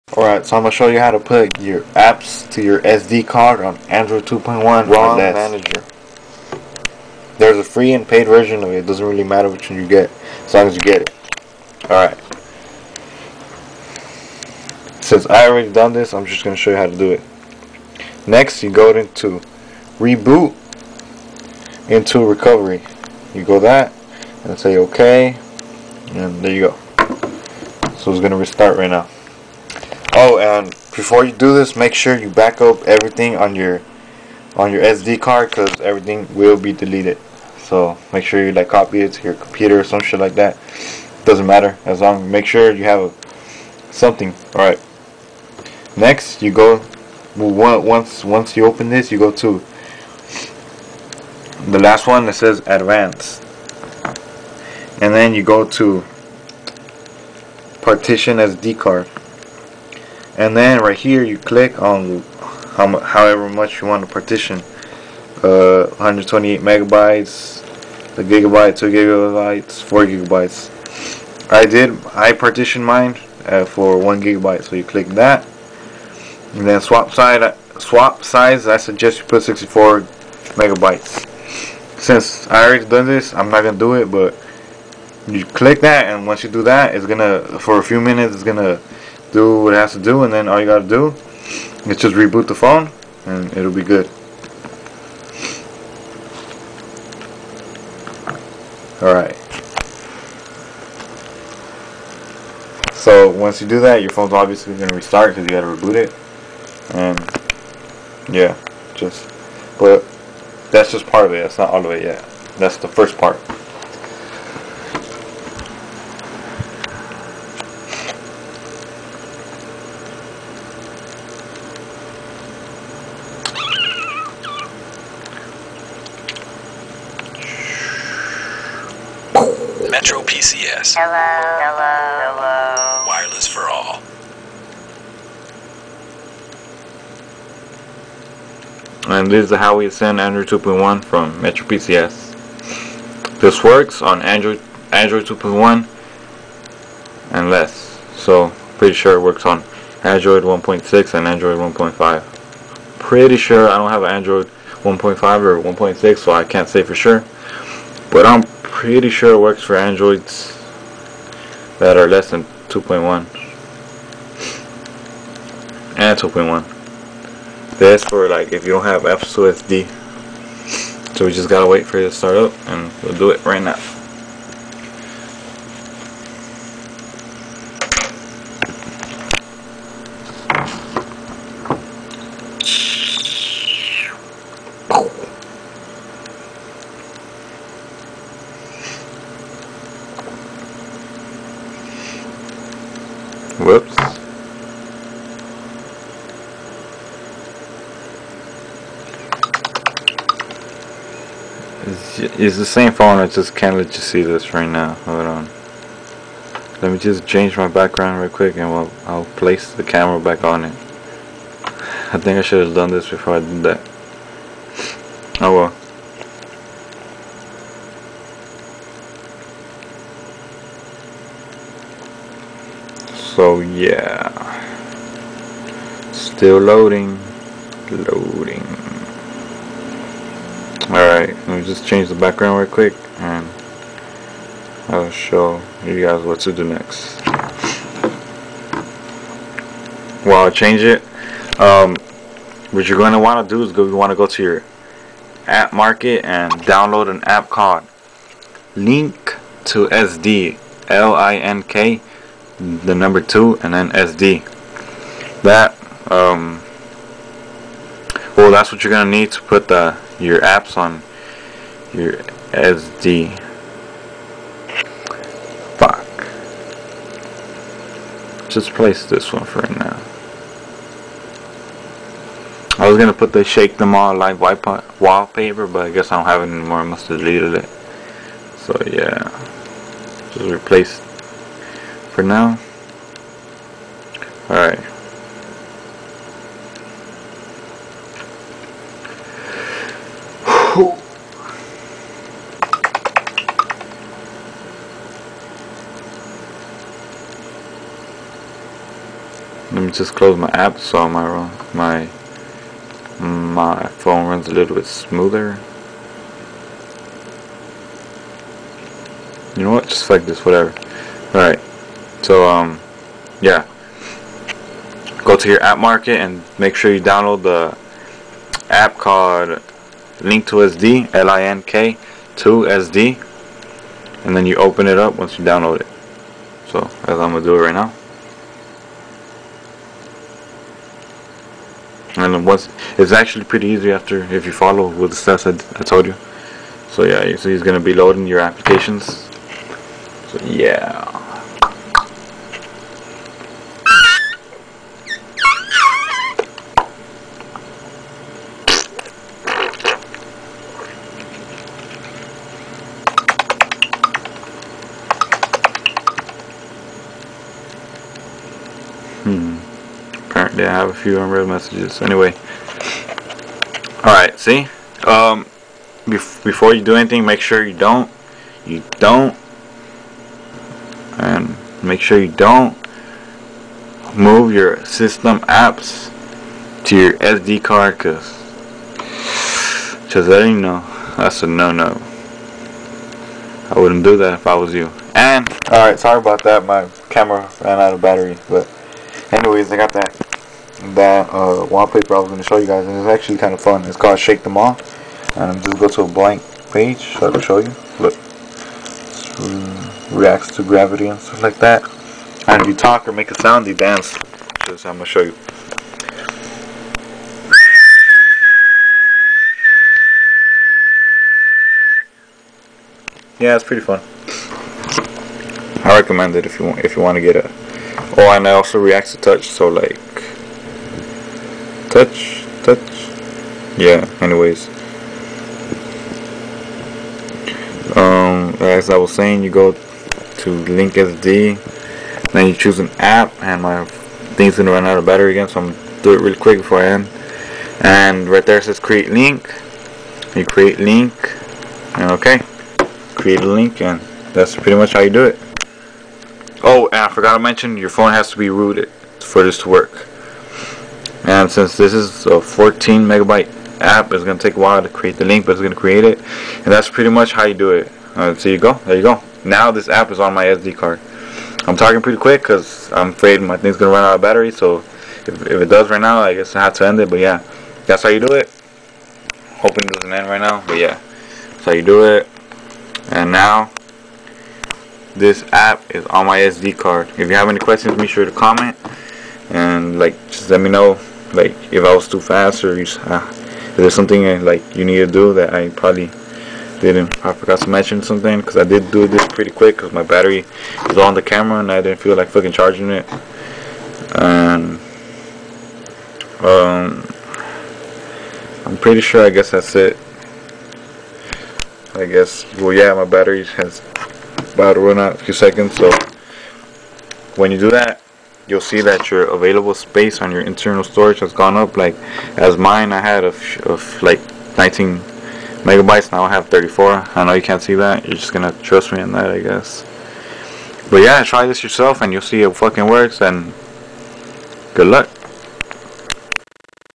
How to move Apps to SD card on android 2.1 and under
This is actually quite easy. This video simply shows you how to move you apps2sd on your android phone if you have android 2.1 or less .. Also your android phone must be rooted ! This is my phone the Huawei Ascend M860 from Metro PCS.

apps you will need are: both available in market place
rom manager
link2sd